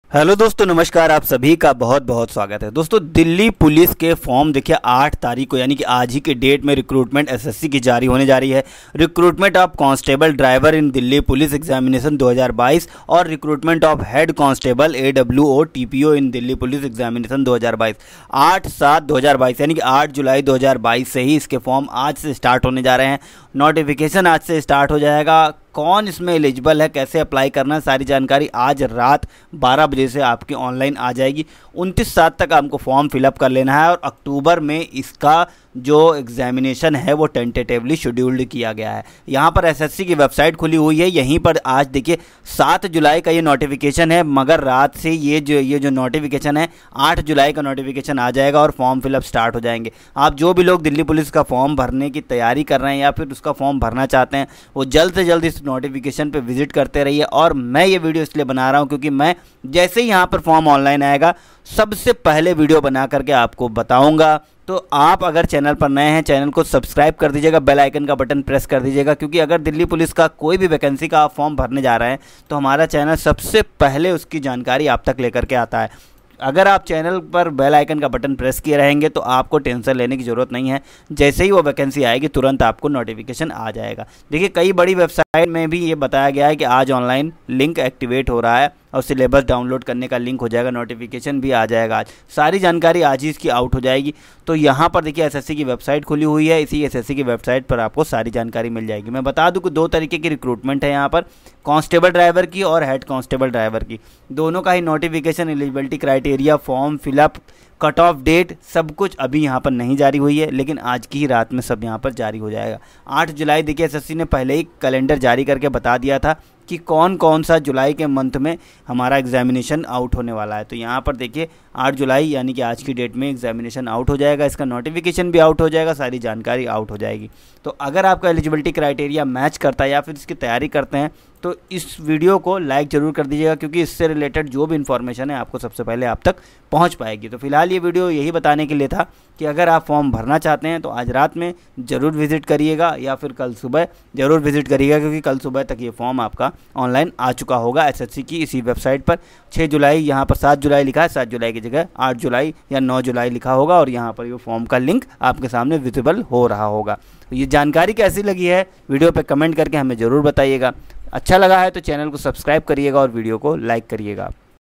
SSC Delhi Police Constable / Head constable driver Recruitment 2022 Online Form
हेलो दोस्तों मै  इस विडियो में " SSC Delhi Police Constable / Head constable driver Recruitment 2022 Online Form"से सम्बंधित कुछ बहुत ही अहम् जानकारिया देने वाला हूँ।  आप लोग इस वीडियो को पूरा देखे और इस तरीके से फॉर्म अप्लाई करें।।

FULL NOTIFICATION LINK👇
updating soon

APPLY LINK👇
upadating soon